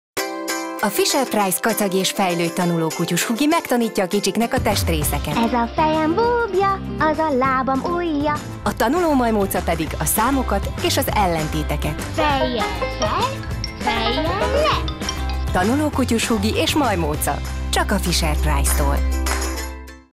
Fischer Price: Tanuló Kutyushugi és Majmóca- REGIO JÁTÉK
Leírás: Tanuljatok együtt nevetve a Fisher-Price Tanuló kutyushugival éa majmócával! Az állatok testrészeit megnyomva a játék megtanítja azok neveit, a színeket, az ABC-t és a formákat is.

Kövess bennünket Facebookon is!